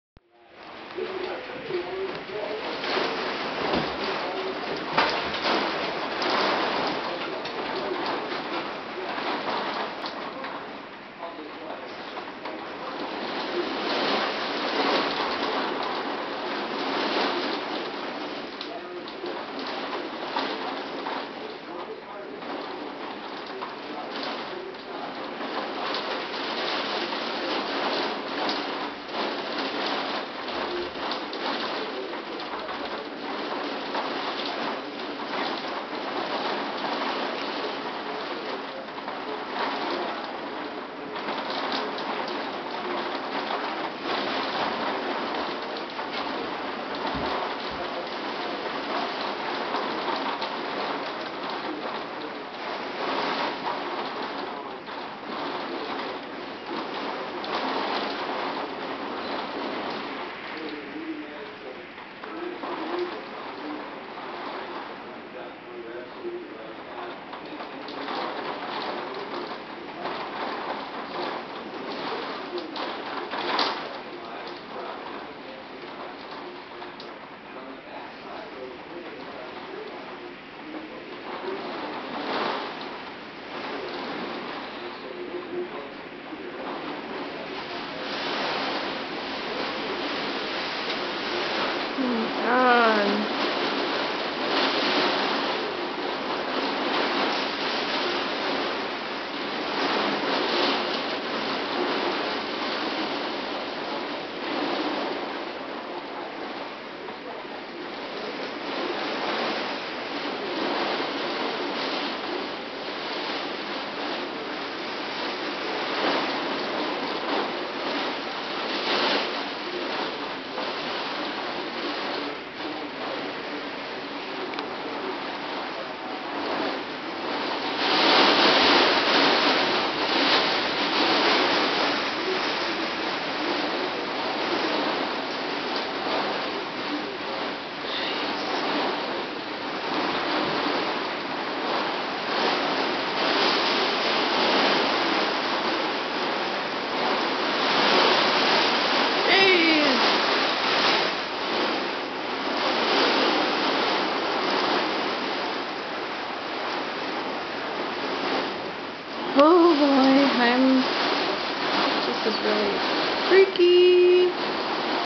Thunderstorm2 6-21-13 North St Paul
Huge storm that hit last night with 50-70mph winds, torrential rain fall and hail. Over 2 inches of rain in less than an hour.